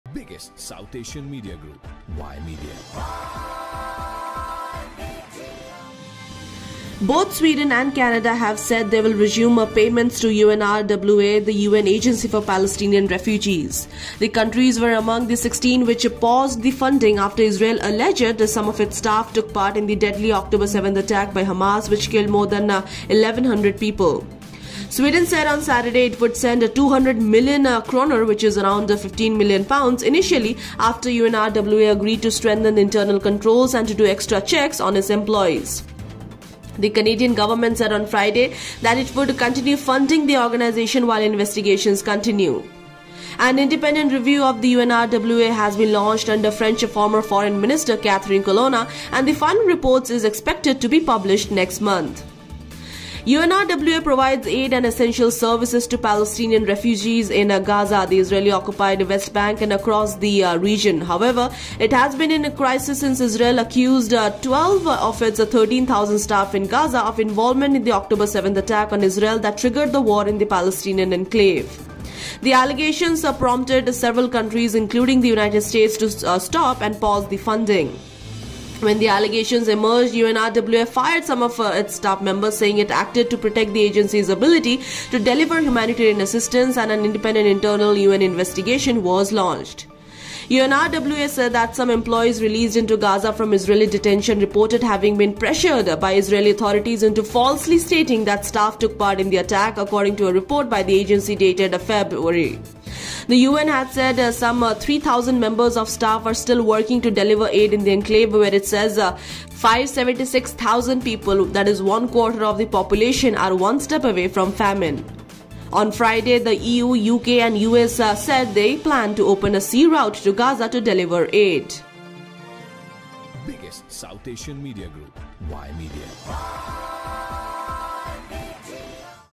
Sweden and Canada Resume Funding For Un Agency For Palestinian Refugee
Y Media Radio Y LIVE Now 24x7